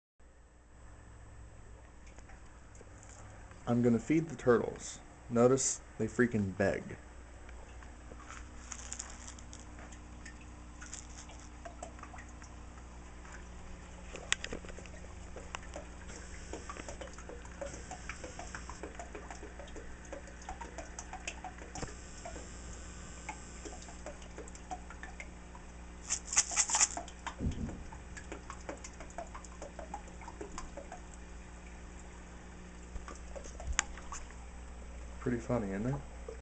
begging turtles
my three turtles beg for food and make a "splash" XD pun intended.